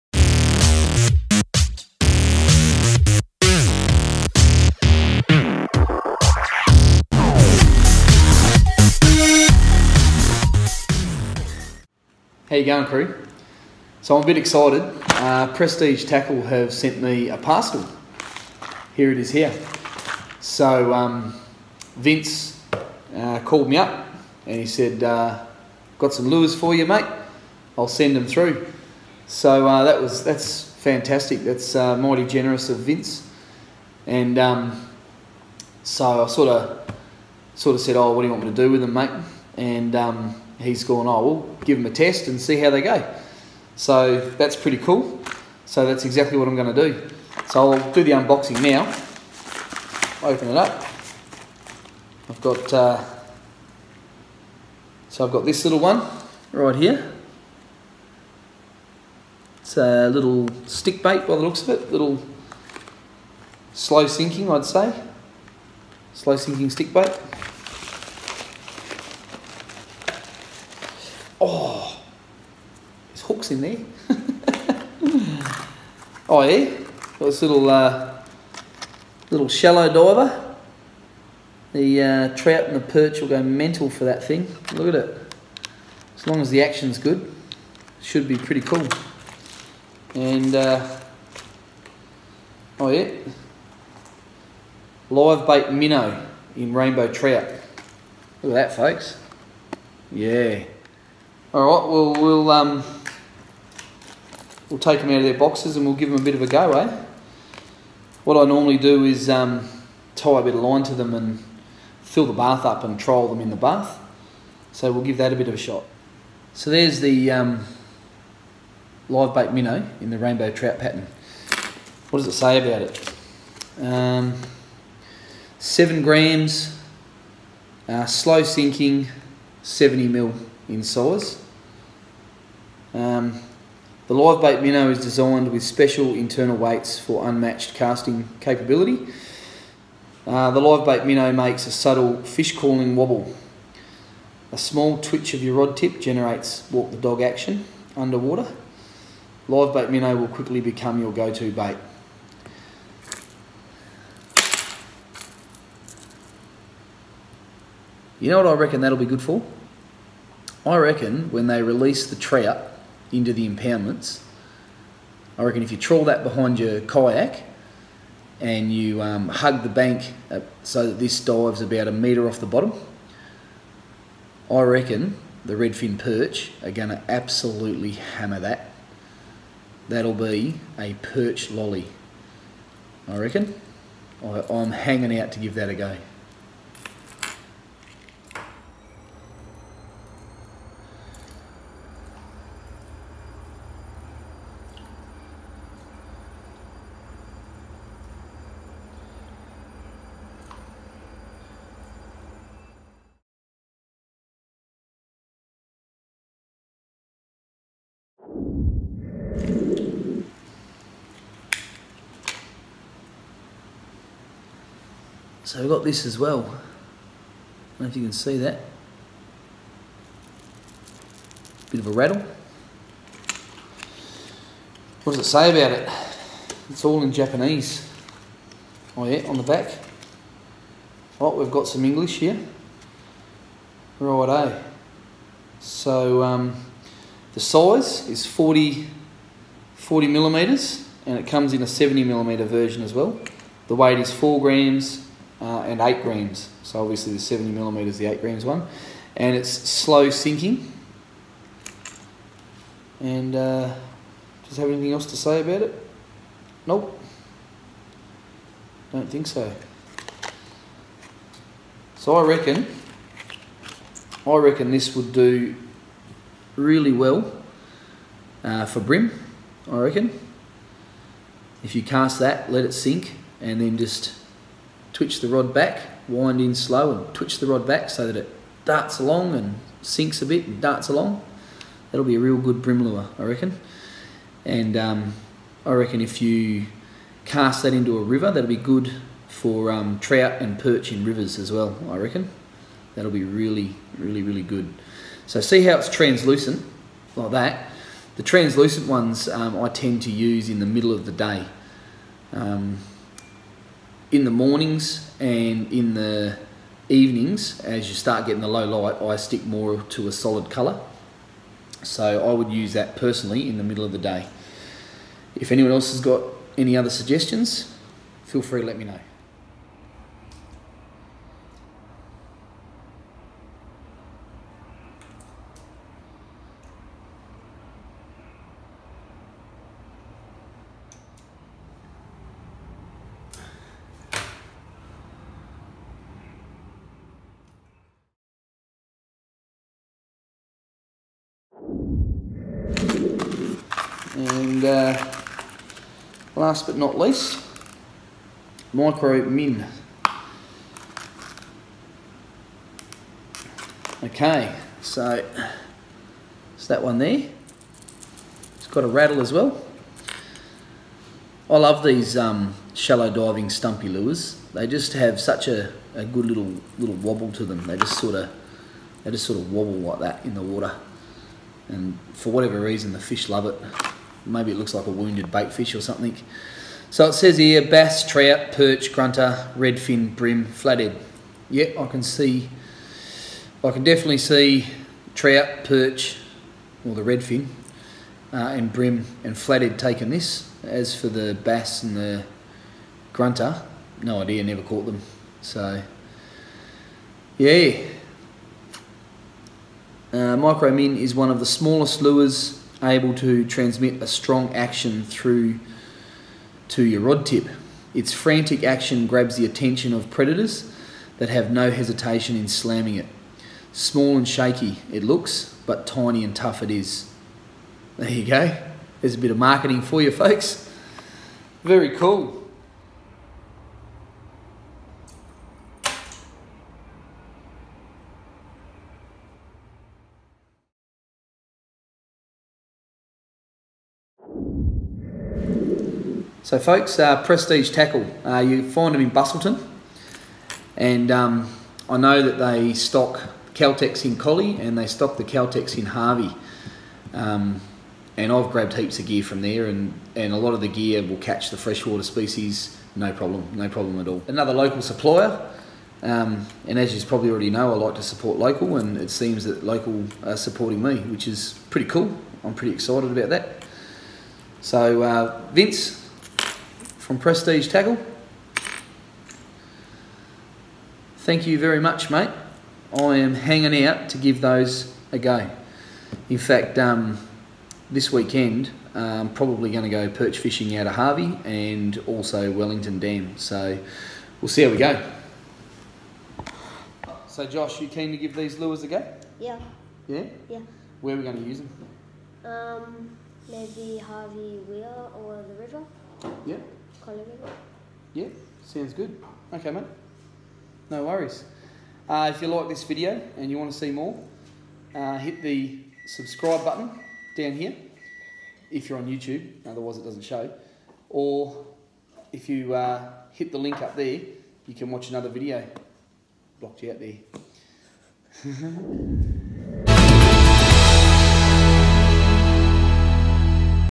My First Unboxing - Lures From Prestige Tackle
So this is the 3rd package that Prestige Tackle has sent me, the first 2 contained Micro Spoons and then standard Spoons. I was successful at pinning Redfin Perch with all of them, unfortunately battery power was an issue and I was unable to capture any decent footage.
I am hoping to get a good opportunity this weekend 7-8/7/18 to hit the Harvey Weir and make another vid about how all the gear performed.
I love supporting local as I know any purchase I make goes to great hard working local members of the community. I feel so blessed that Vince has sent me a few packages and I am glad I can support him by making some product review vids.

I hope you find my vids entertaining, I always try to leave a few little bloopers in there to entertain LOL.
I would ask any USA crew who fish for Bass to let me know what they think of the lures and how they think they would perform on Bass.

Thanks crew and tight lines.